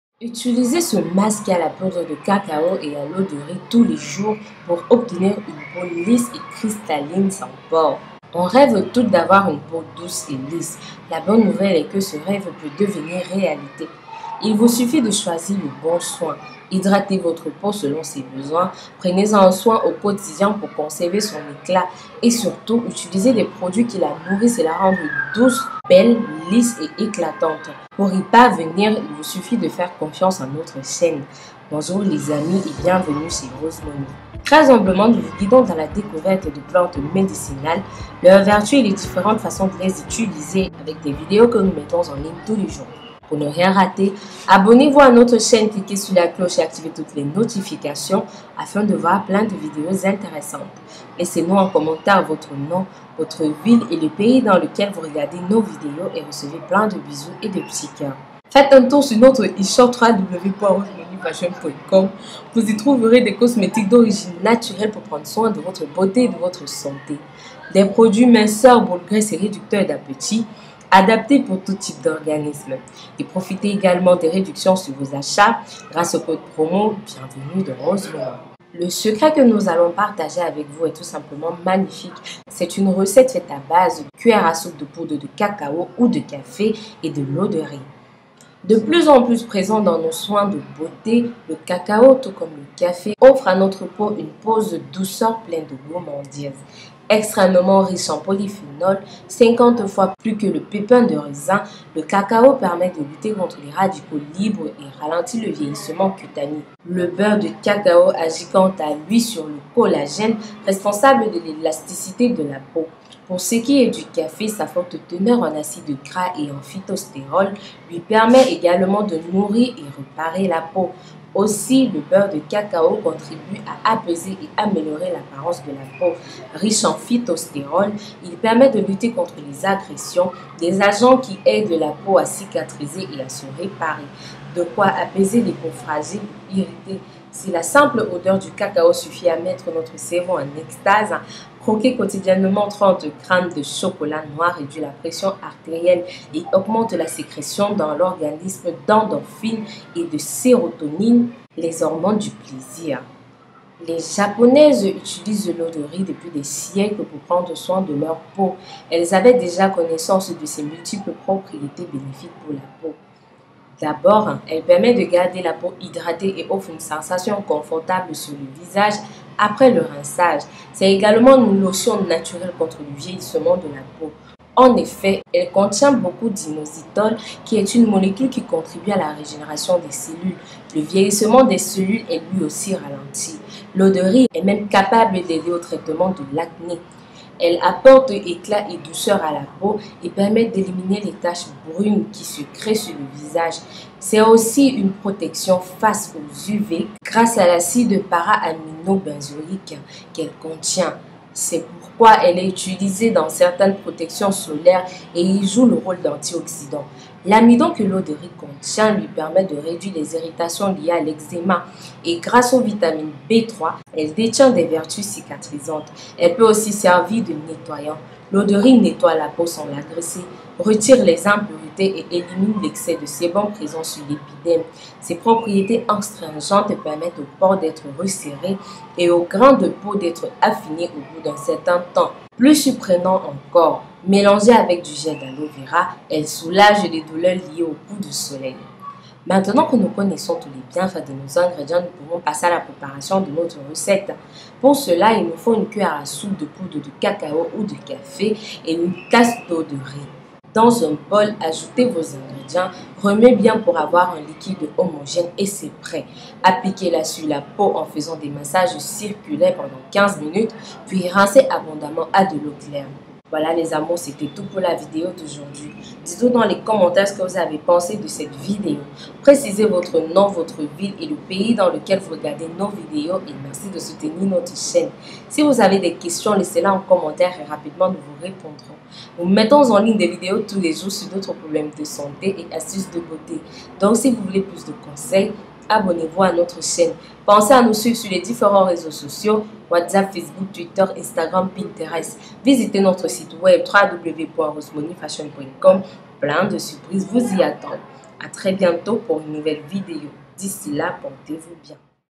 Utilisez ce masque à la poudre de Cacao & à l'eau de riz tous les jours, pour obtenir une peau lisse
Utilisez ce masque à la poudre de Cacao et à l'eau de riz tous les jours, pour obtenir une peau lisse

Ingrédients: une cuillère à soupe de poudre de cacao ou de café et de l'eau de riz.

Préparation: Dans un bol, ajoutez vos ingrédients, bien remuer pour avoir une pâte homogène et c’est prêt.

Posologie: Appliquez sur la peau en faisant des massages circulaires, puis rincez.